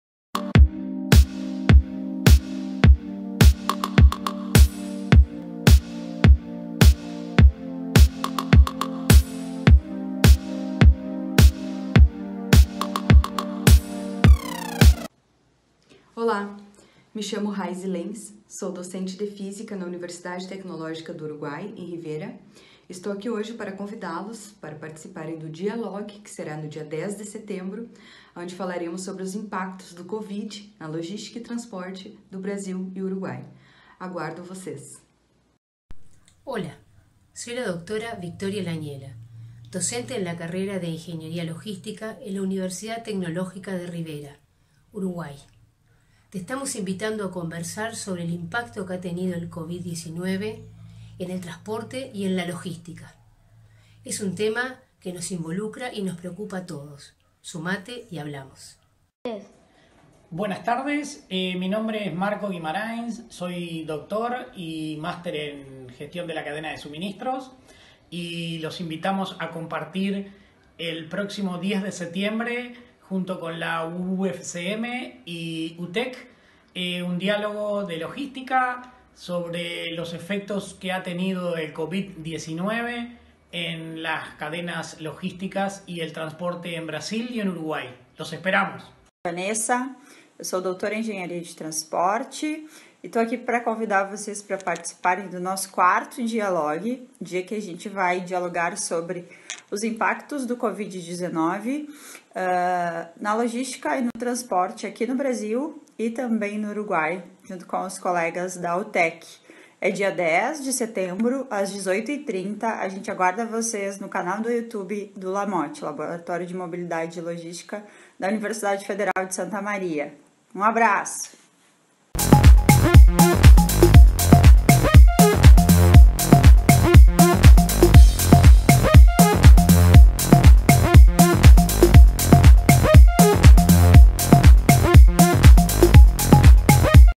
Convite para o 4º DiaLOG
O LAMOT apresenta a todos o convite dos participantes do 4º DiaLOG, que será realizado no dia 10 de setembro, às 18:30! Não percam essa oportunidade de entender um pouco mais sobre os impactos da COVID-19 na logística e transportes no cenário internacional!